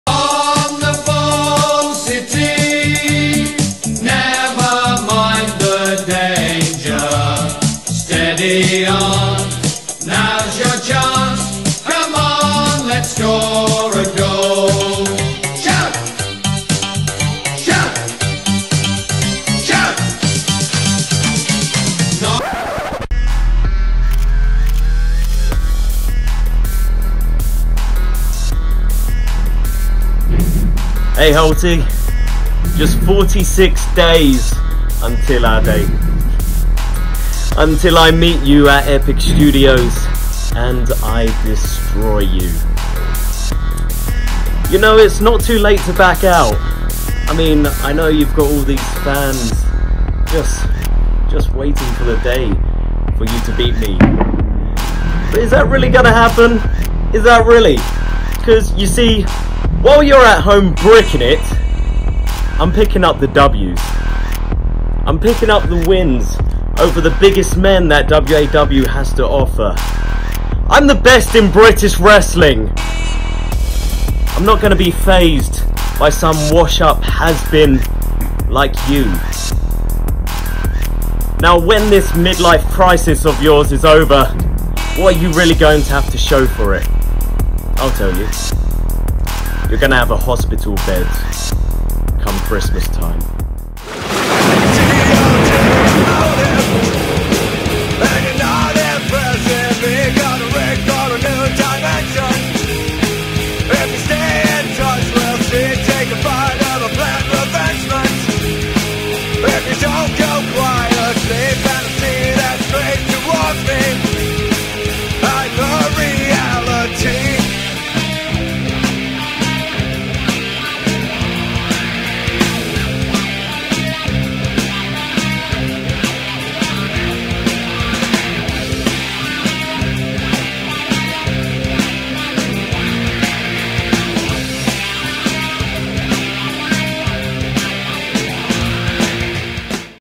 Brad O'Brien vs Grant Holt Promo
Brad O'Brien faces his toughest challenge as Ex Norwich City Striker Grant Holt will face off in his singles debut for WAW in December. Can Holt Dethrone the King of Couture?